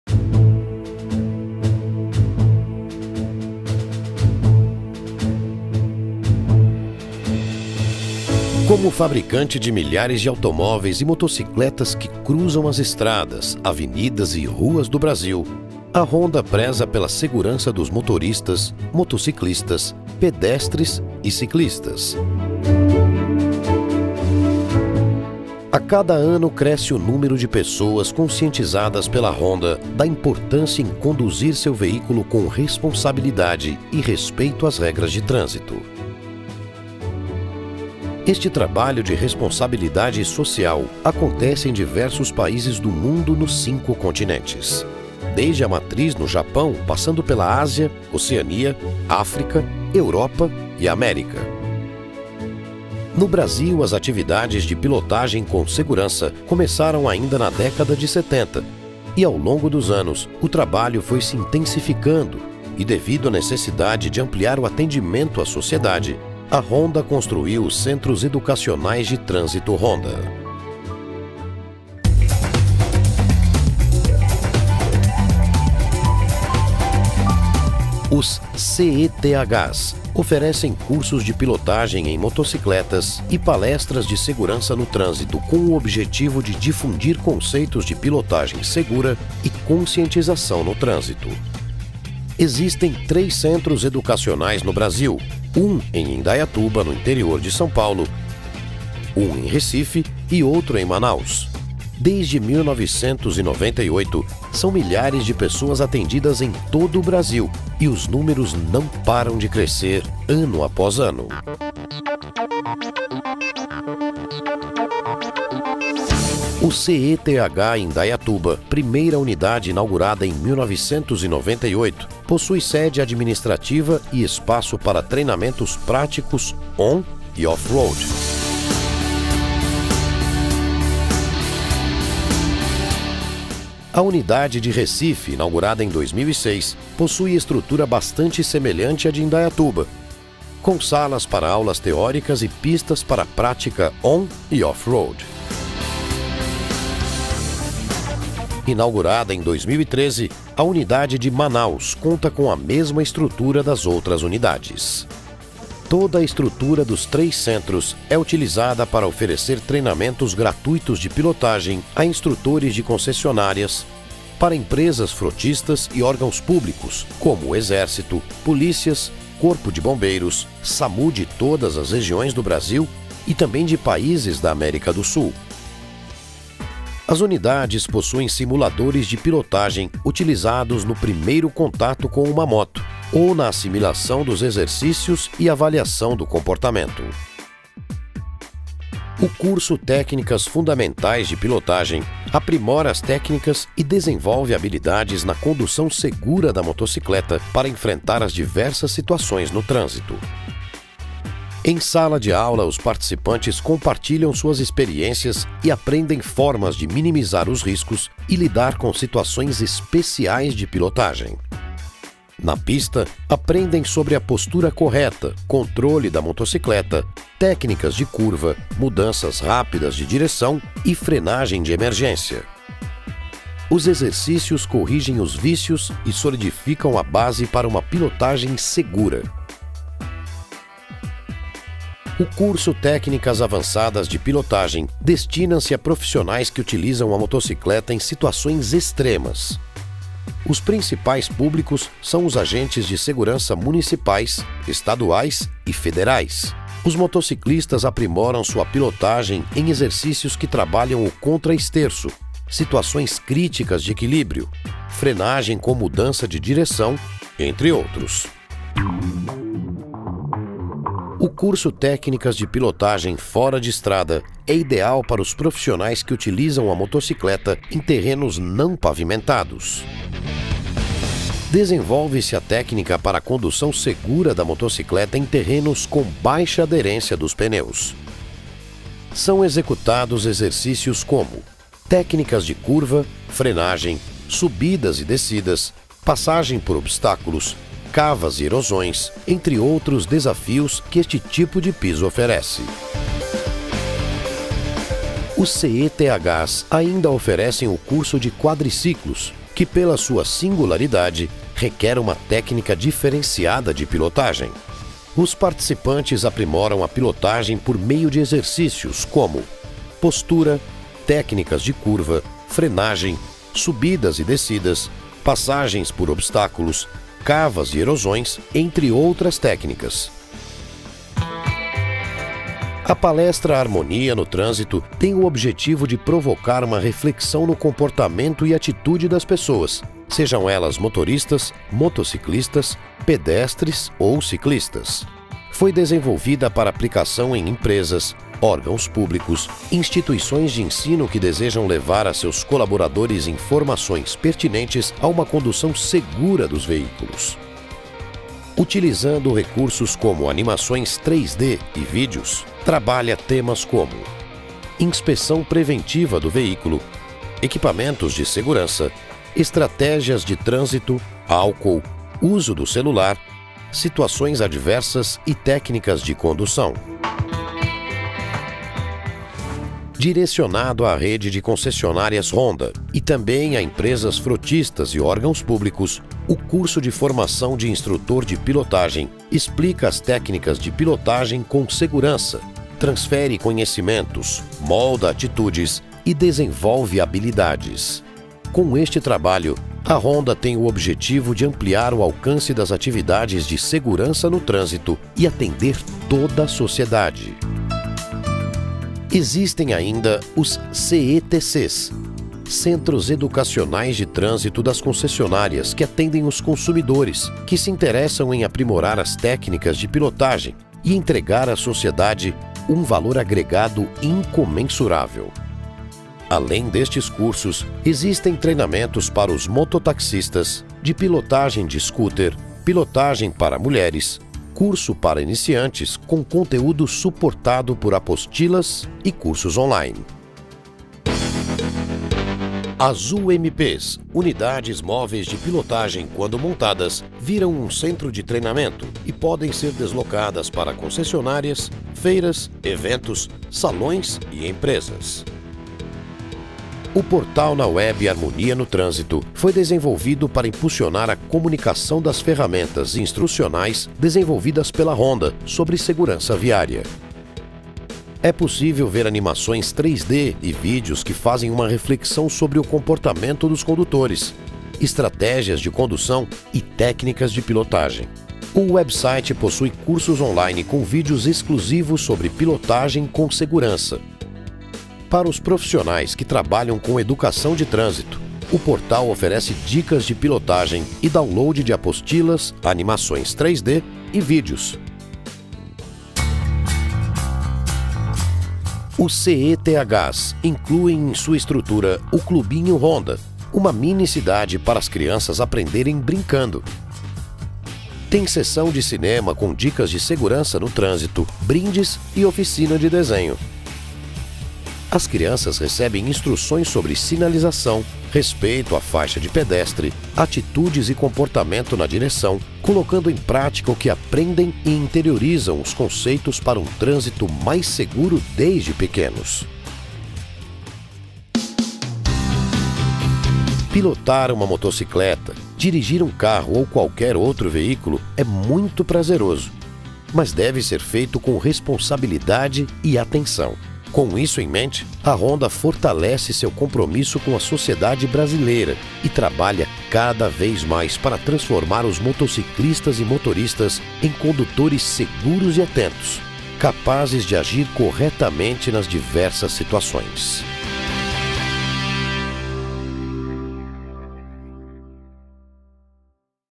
Iniciativas Honda
A ideia de direção responsável começa lá na matriz da Honda, no Japão, e se espalha pelos cinco continentes do mundo. Você também pode fazer a sua parte para um trânsito mais consciente. ;)